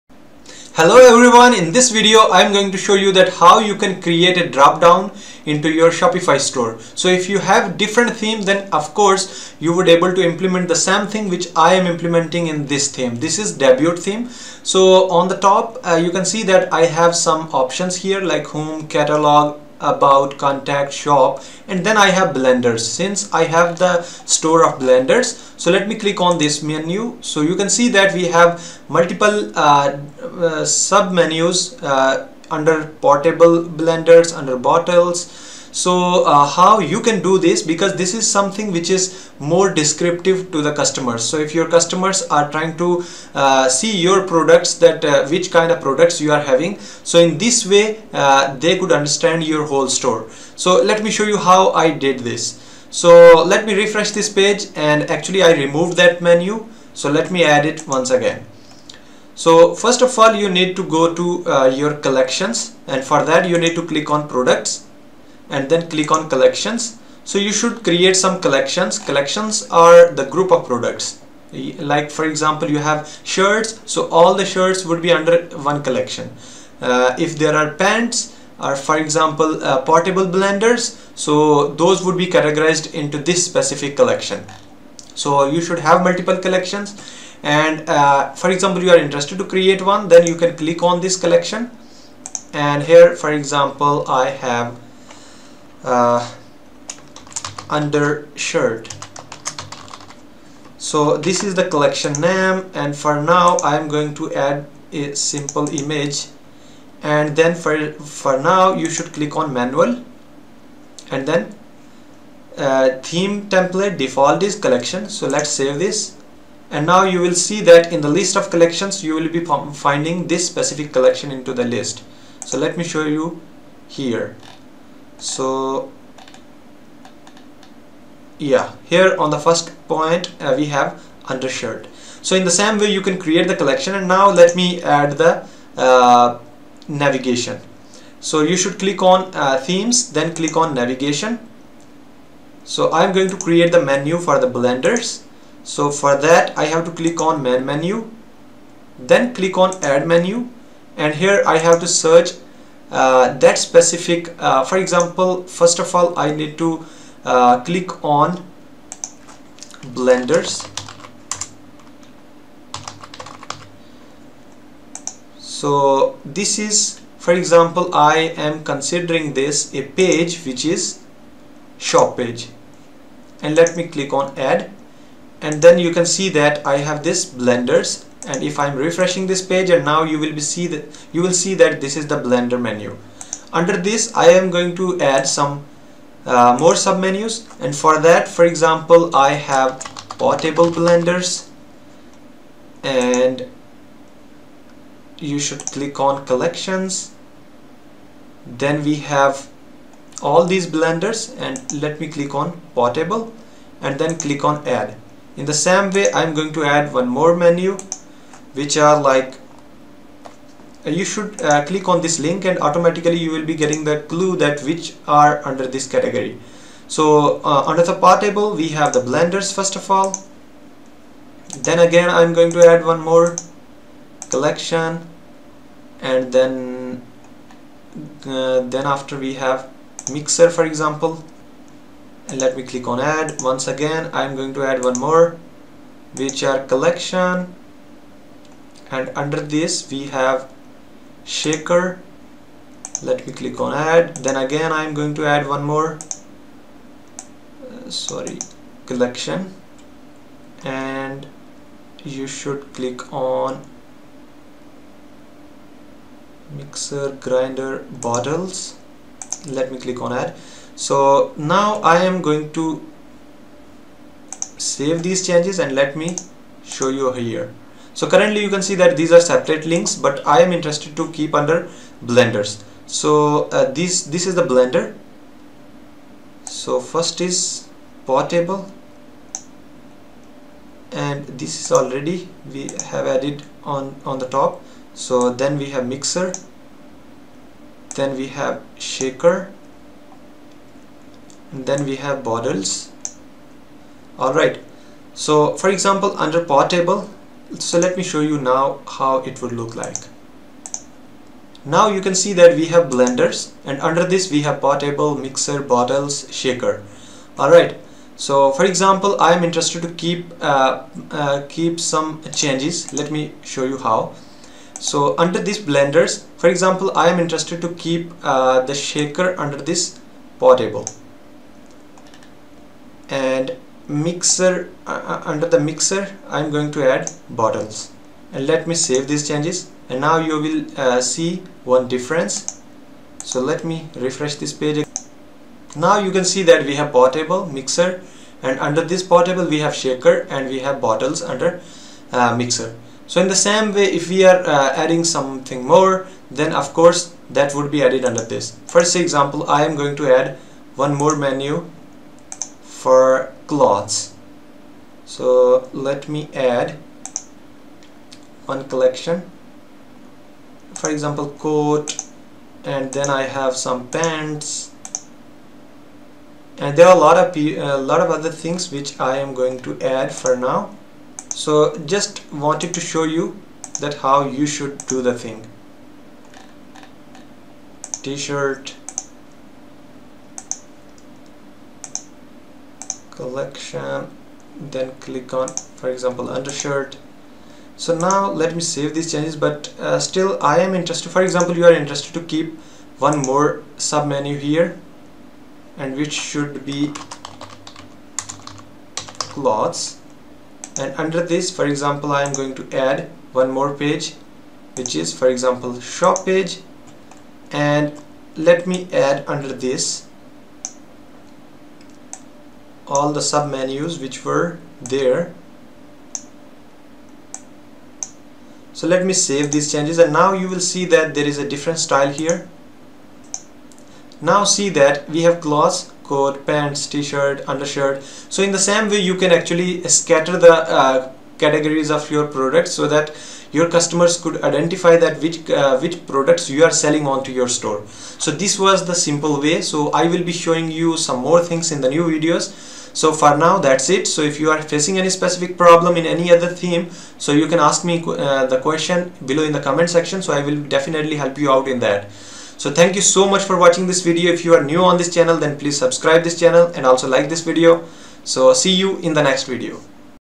🛍️ How to Create Dropdown Menus in Shopify Store | Help Customers to See Your Products
👋Skype: mohdramzaan112